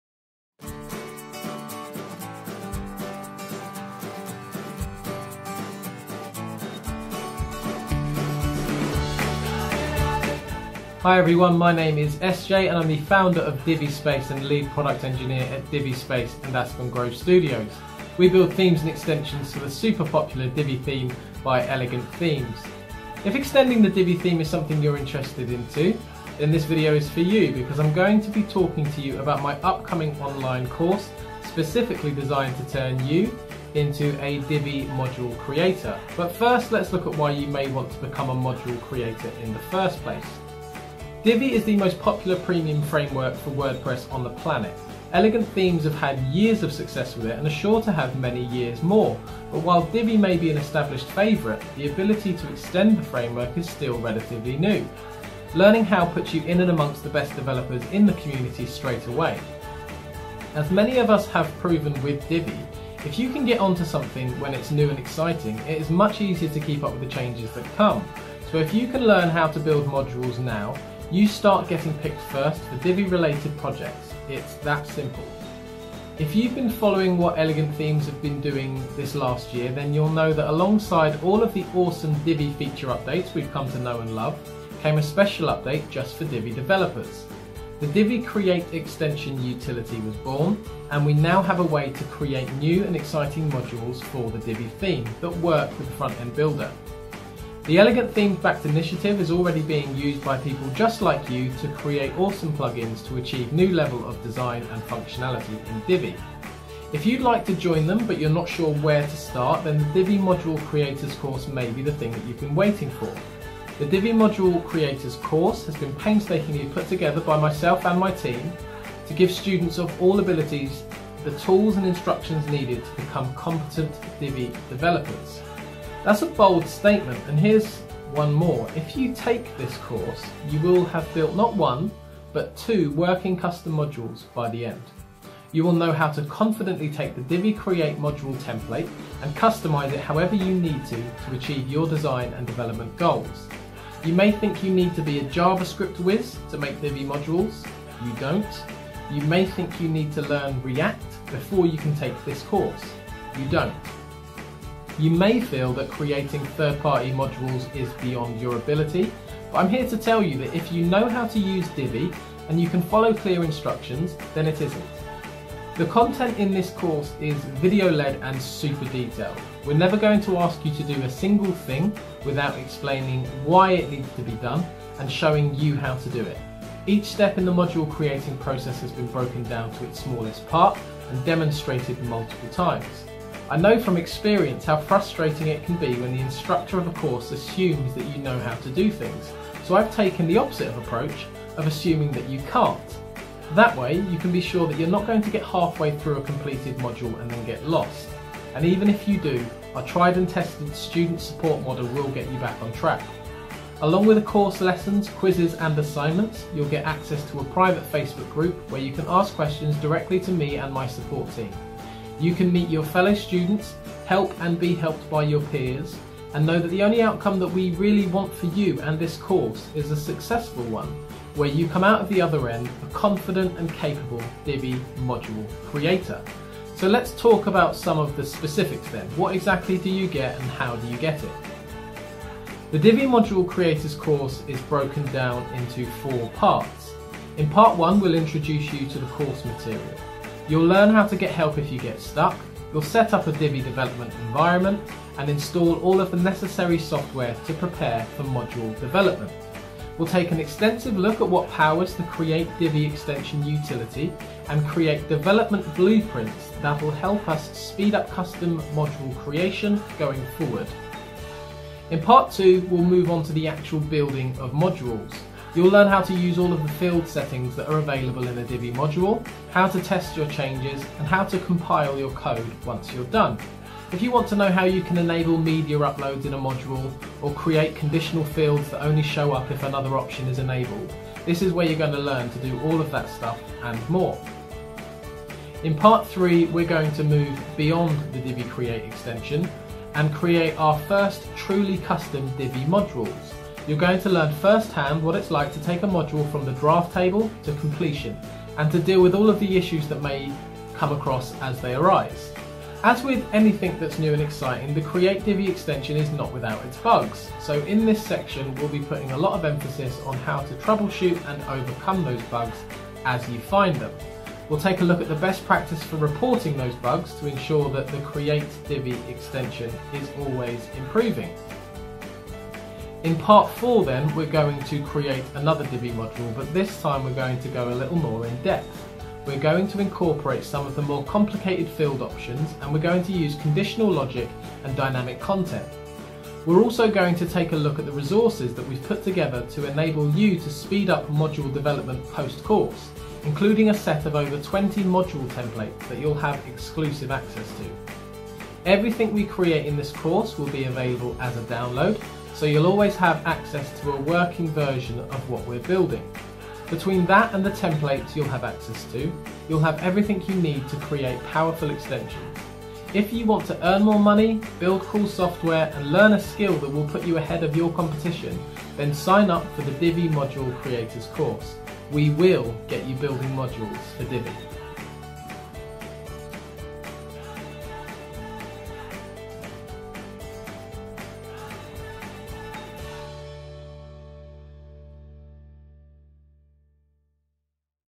How to Become a Divi Module Creator | Divi Module Creators Course
Are you interested in becoming a Divi Module Creator on the Divi Theme? The Divi Module Creators Course is for you!

Instructed by SJ James, the Divi Module Creators Course is for anyone looking to join the growing number of developers who are extending the Divi framework in more creative ways.

In the Divi Module Creators course, you'll learn:

How to set up a development environment, complete with the necessary software requirements for module development.

How Divi Modules work and how they’re rendered on the frontend.

How to create modules using the Create Divi Extension utility.

How to use React.js and JSX for your Divi development purposes.

How to troubleshoot any issues that may arise while you’re building Divi modules.

And that's not all! By the end of the course, you will have 2 working custom modules!

Stay up to date with the latest WordPress news and our product releases!